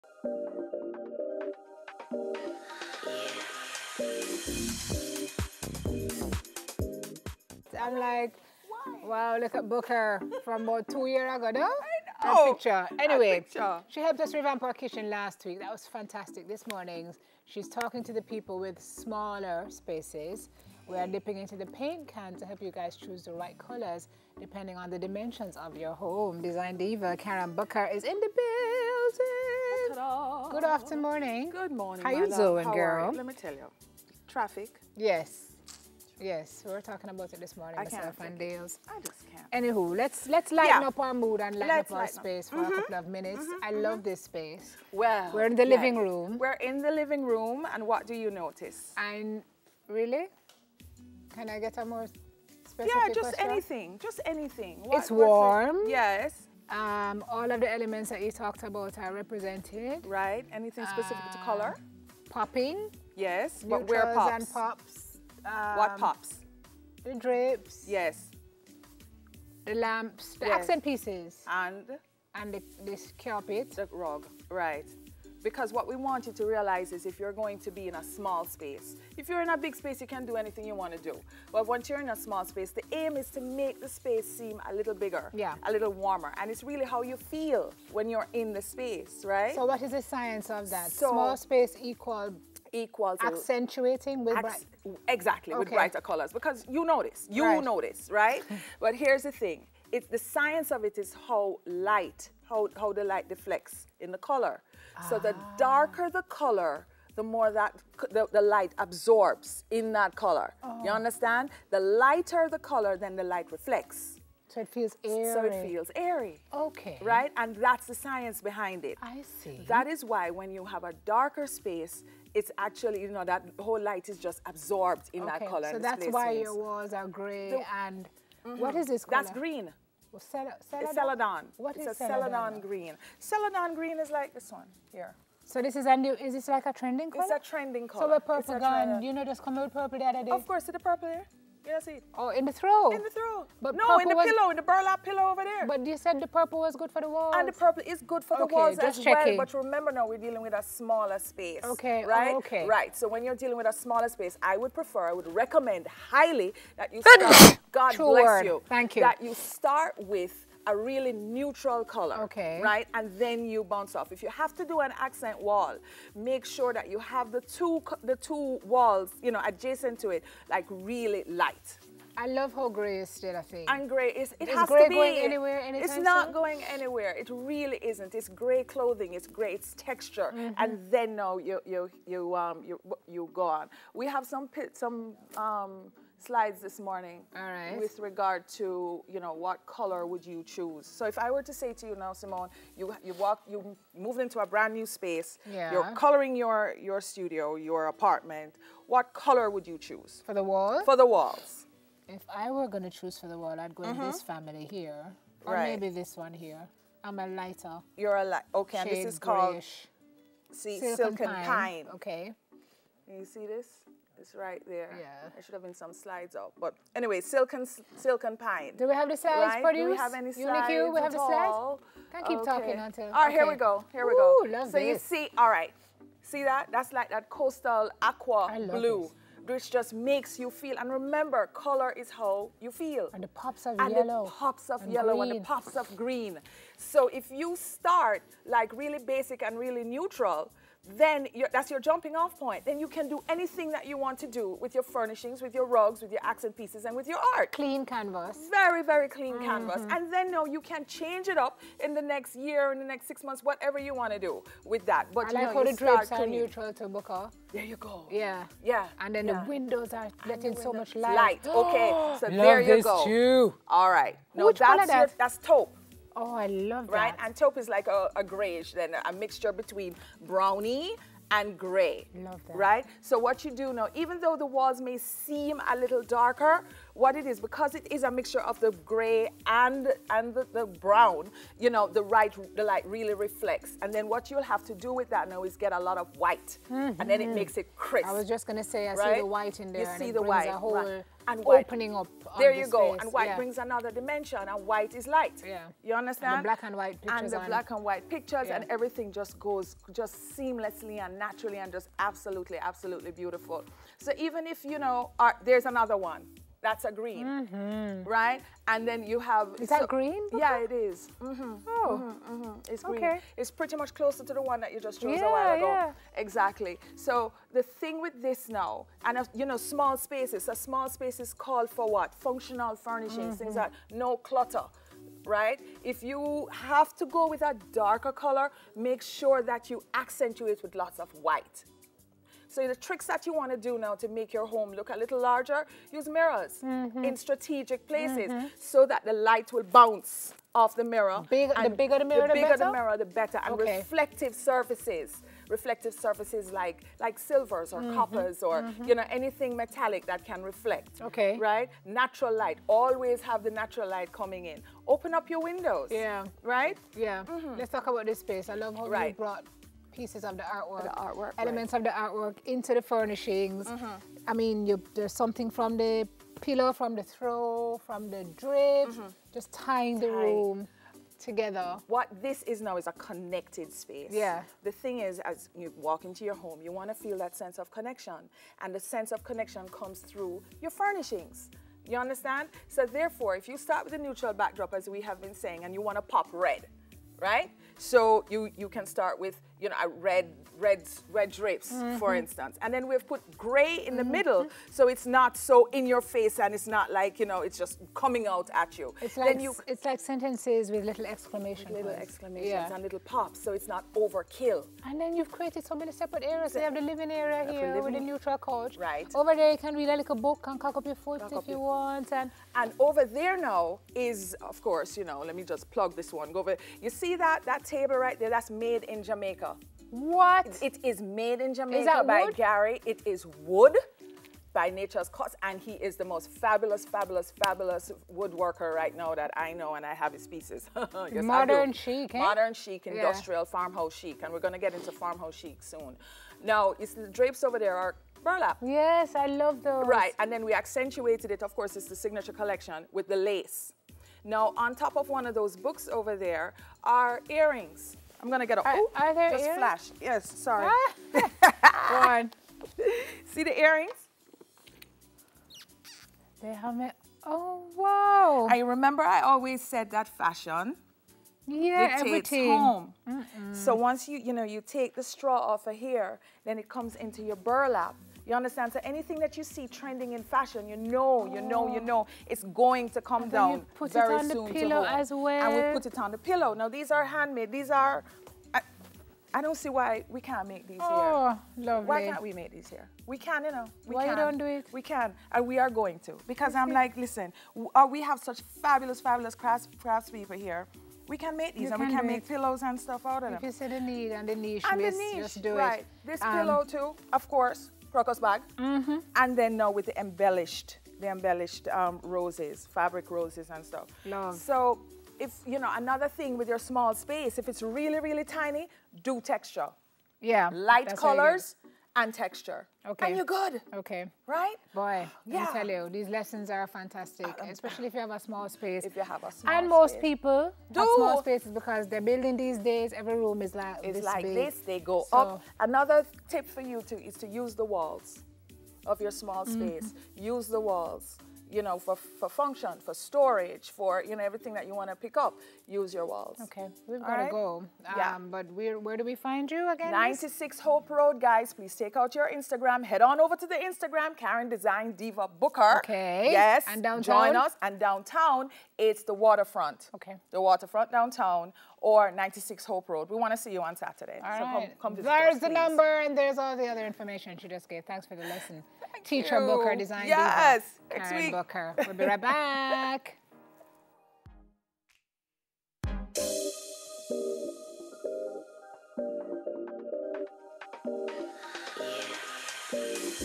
DeZign Diva talks about the Best Colours for Your Small Space
DeZign Diva talks about choosing the best colours for your small space